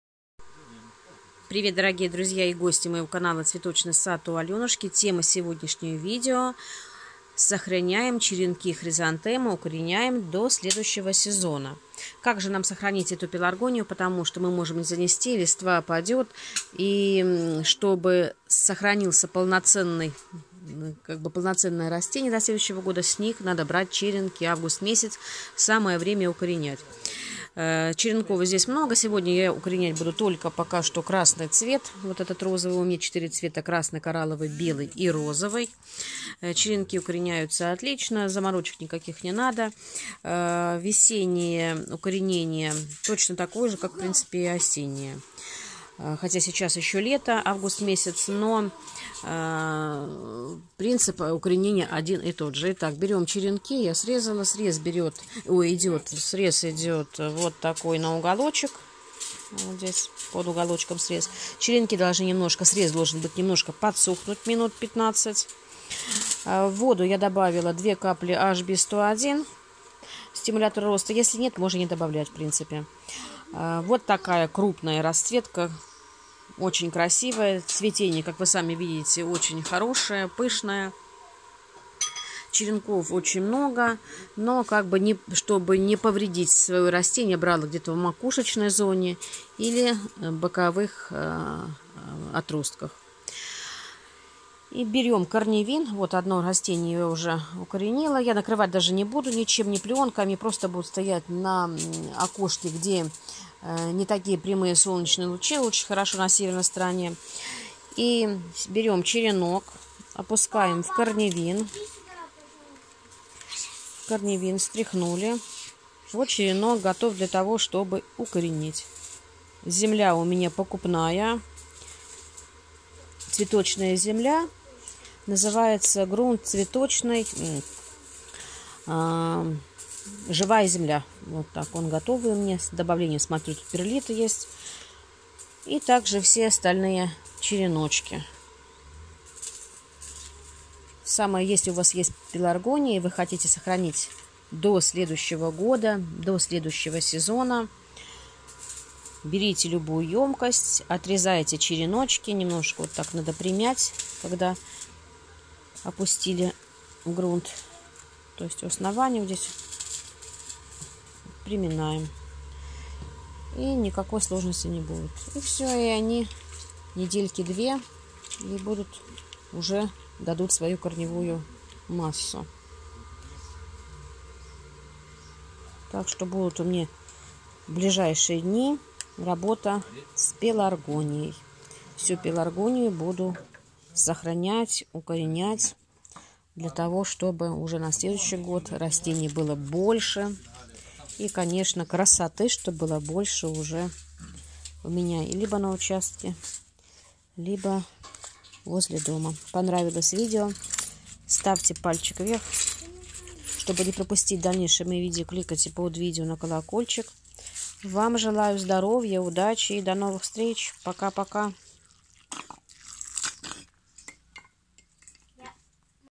ЧЕРЕНКОВАНИЕ ПЕЛАРГОНИИ В АВГУСТЕ.СОХРАНЯЕМ РАСТЕНИЕ ДО СЛЕДУЮЩЕГО ГОДА.
УКОРЕНЯЮ ЧЕРЕНКИ ПЕЛАРГОНИИ ИЛИ ГЕРАНИИ.ОНИ БУДУТ ВДАЛЬНЕЙШЕМ СЛУЖИТЬ НАМ МАТОЧНЫМИ КУСТАМИ ДЛЯ РАЗМНОЖЕНИЯ В НОВОМ СЕЗОНЕ.